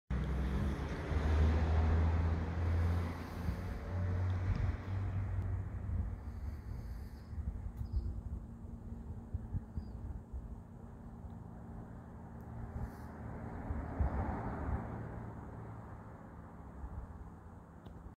My flag of the day 23 April 2021...Isles of Scilly
My flag for today is the flag of the Isles of Scilly which are an archipelago off of the southwestern England.